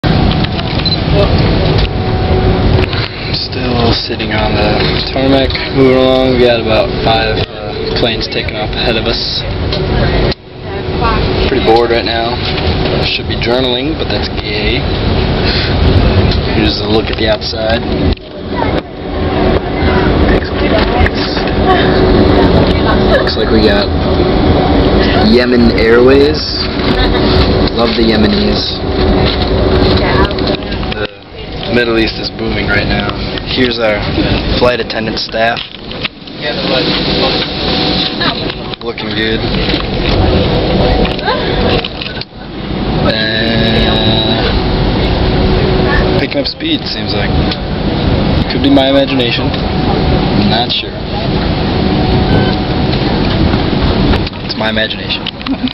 A few words before take-off
a few words with michael david grant before taking off from london to vienna